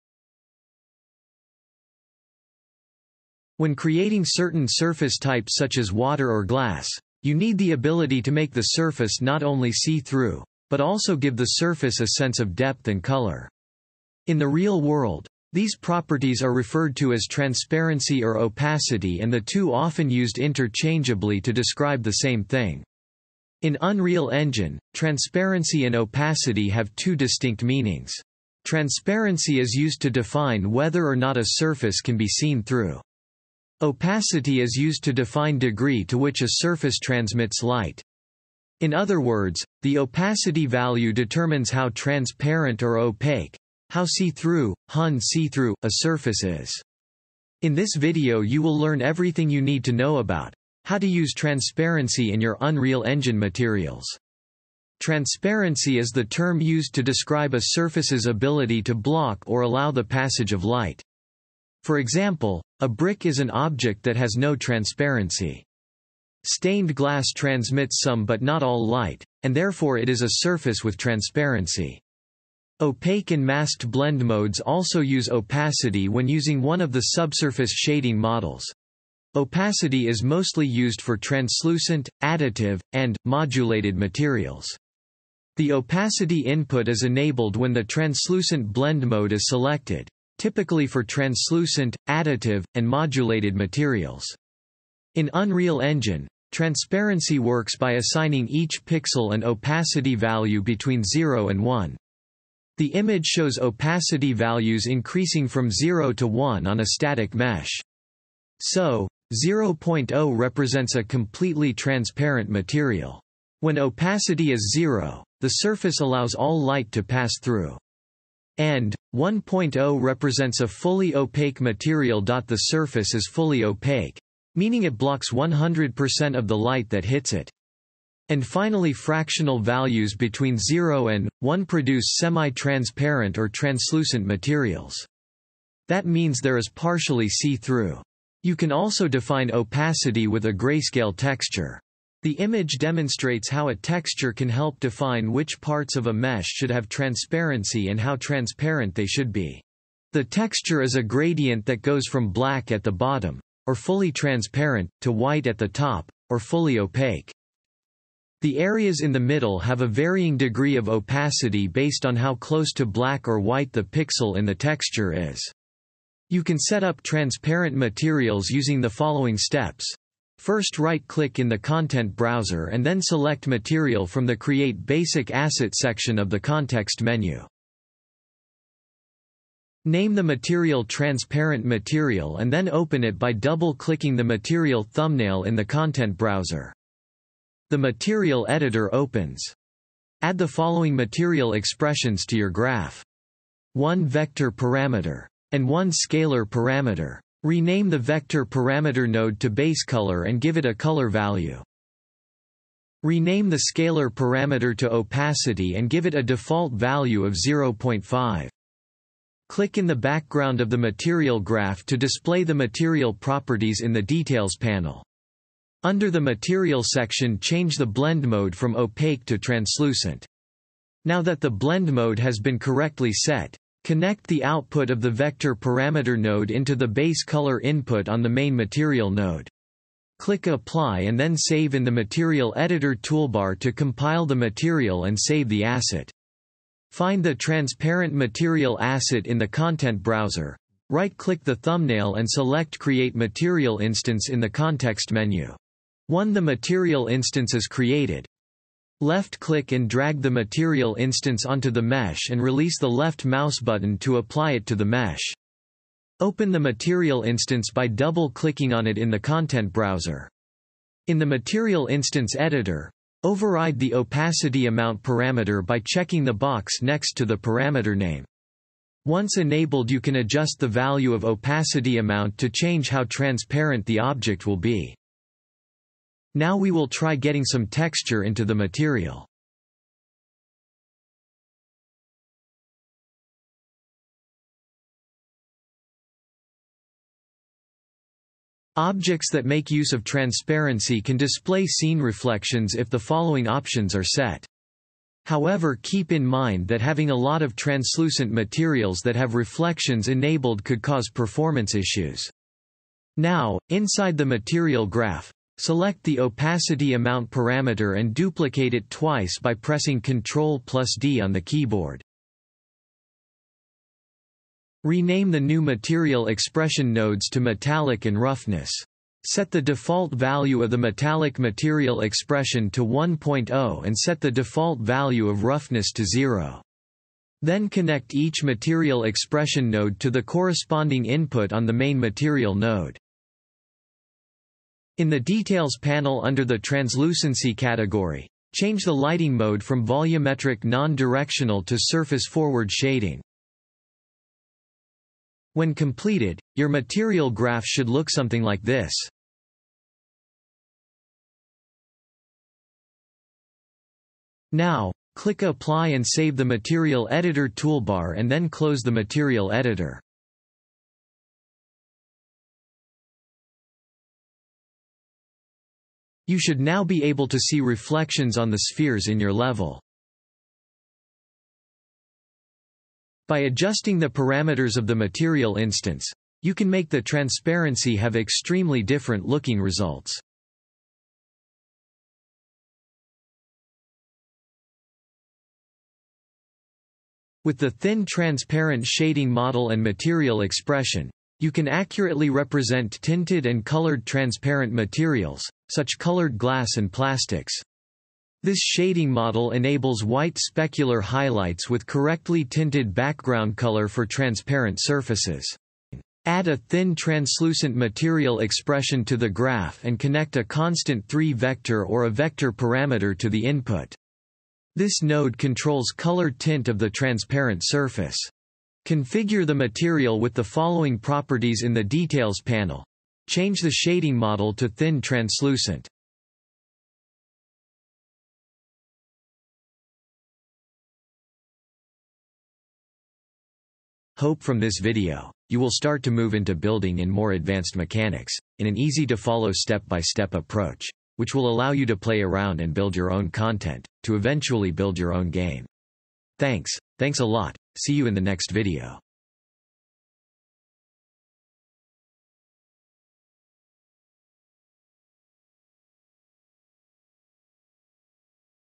UE5 : Materials for beginners in Unreal Engine5- 5 -Opacity & Transparency
This is the 5th video in a  series of Unreal Engine5 demonstrating what is Opacity and transparency.
When creating certain surface types such as water or glass, you need the ability to make the surface not only see through, but also give the surface a sense of depth and color. In the real world, these properties are referred to as Transparency or Opacity and the two often used interchangeably to describe the same thing. In Unreal Engine, Transparency and Opacity have two distinct meanings.
In this video you will learn everything you need to know about, how to use transparency in your Unreal Engine Materials.